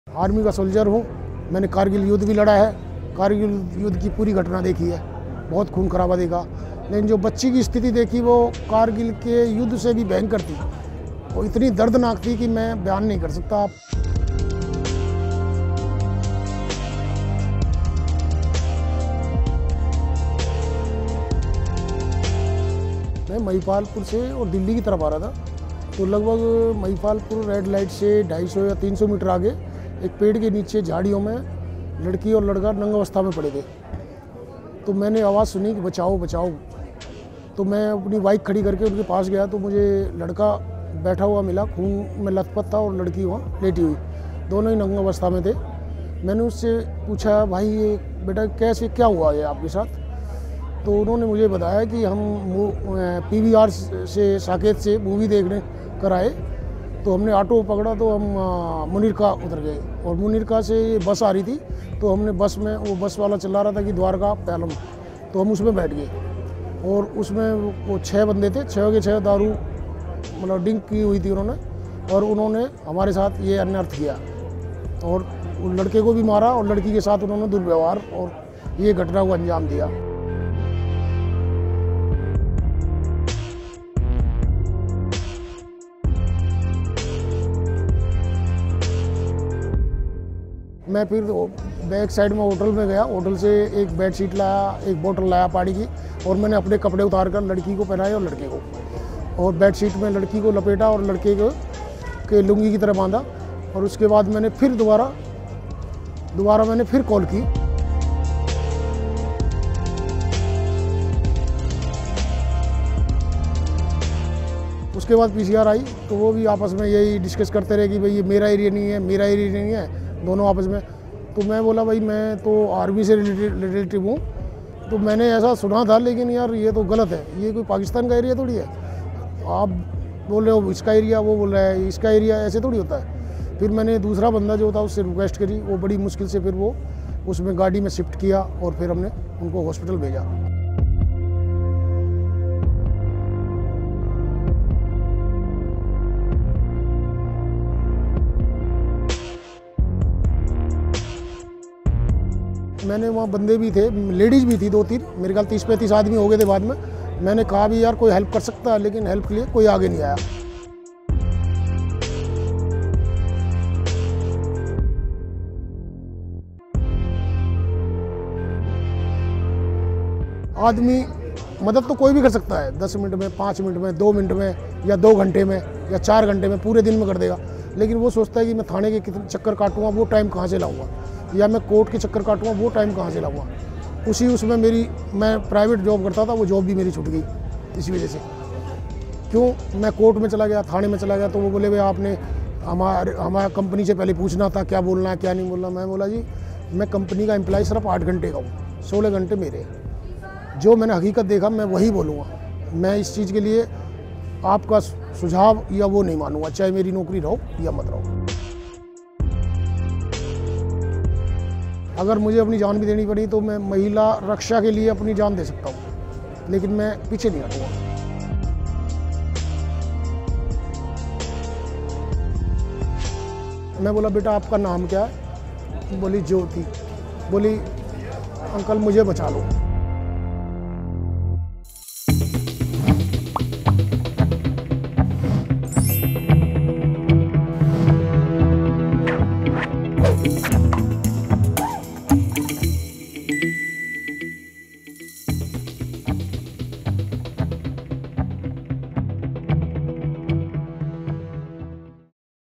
Meet the Man Who Offered the First Help to Nirbhaya 5 Years Ago | The Quint
Rajkumar Singh was the first to help Jyoti Singh and her friend who were lying naked, bleeding in pain on the night of 16 December 2012. It has been five years since the horrific rape case in Delhi shook the nation but Nirbhaya's saviour who had to resign from his job as an aftermath of the police and court proceedings still remains unemployed.